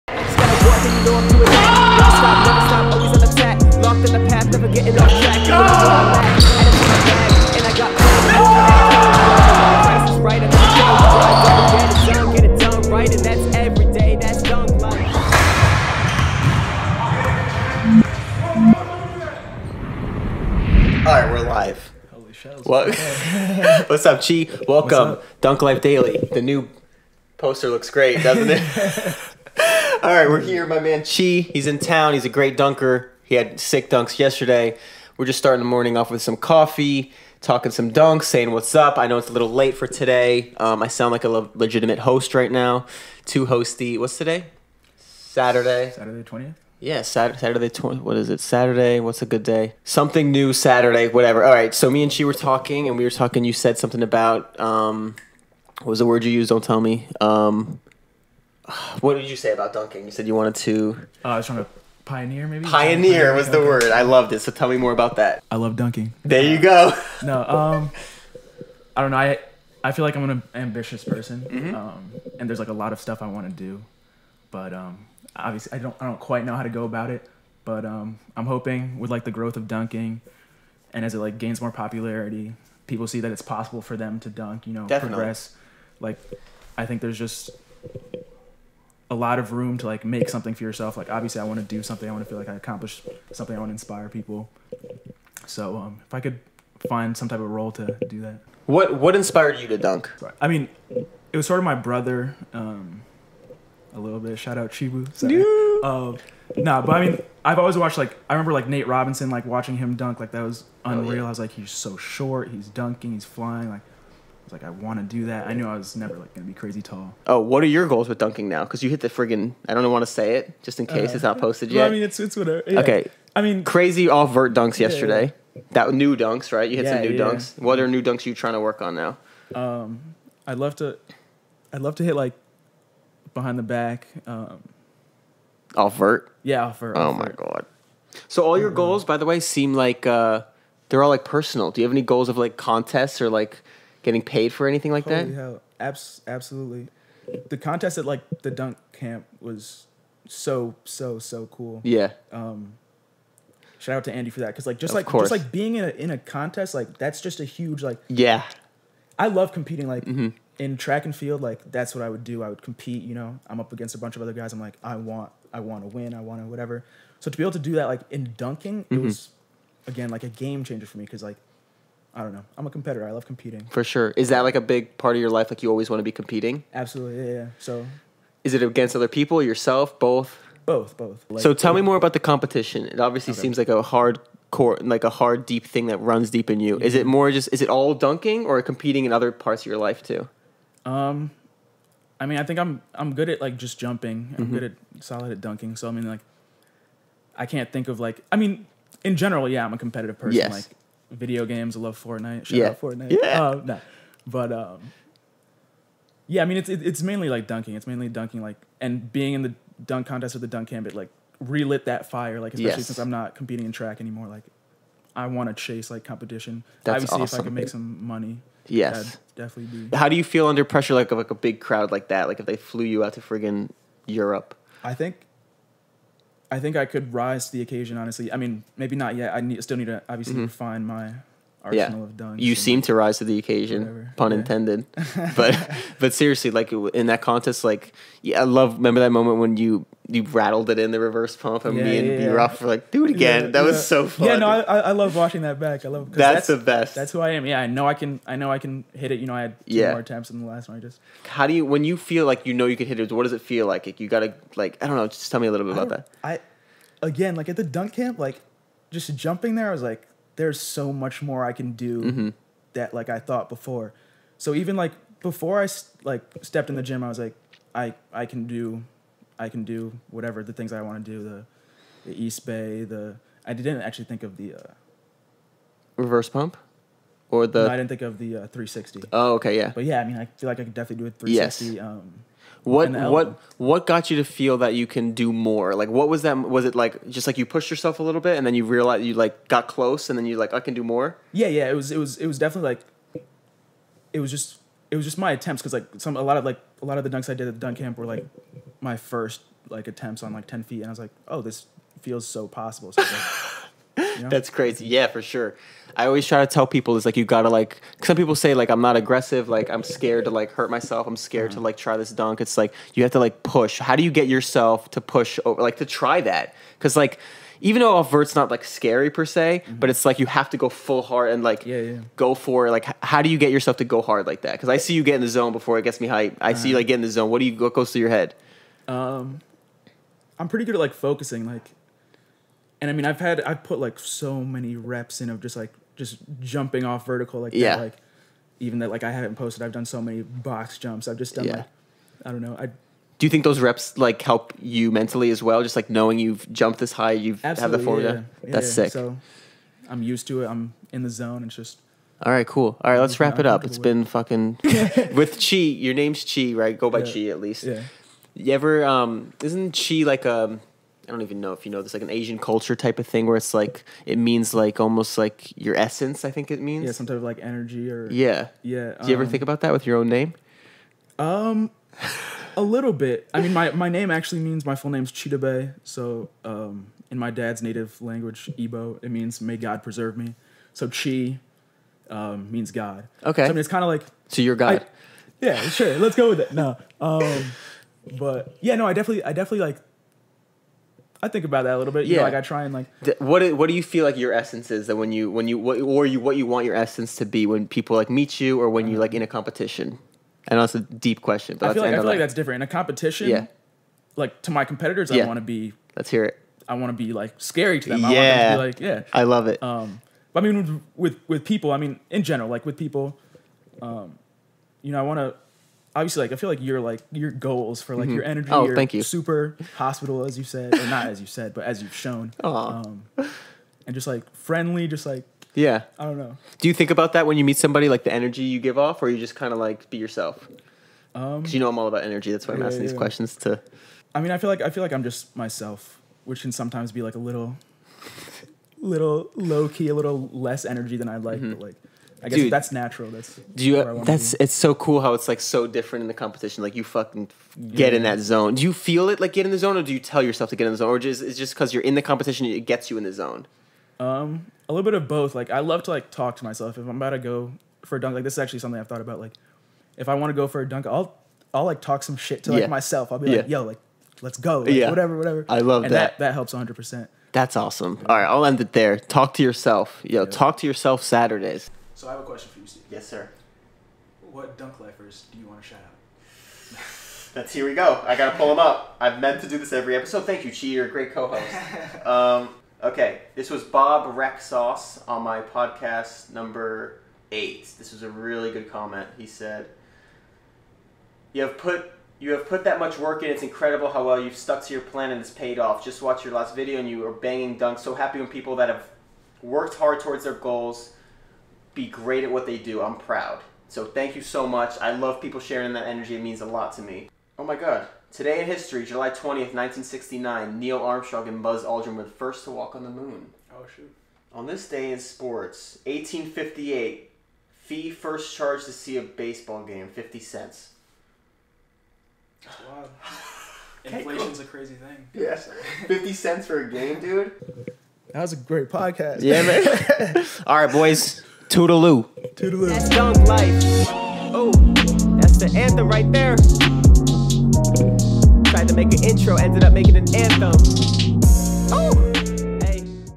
The Energy of Chi! - Dunk Life Daily Ep.11
My man Chi aka DQ (Double Quads aka 4 Legz) joins me (two legz) to talk dunking, competition and getting in the zone! My boy has a 47" running vertical and a standing vert of 42".  The way he focuses and pushes himself to the max always gets me so hype! I see a lot of potential in him and his ambition fuels me to push myself even harder! thats what its all about!! Have a great Self-Talk Saturday and spread the love!

My Gear!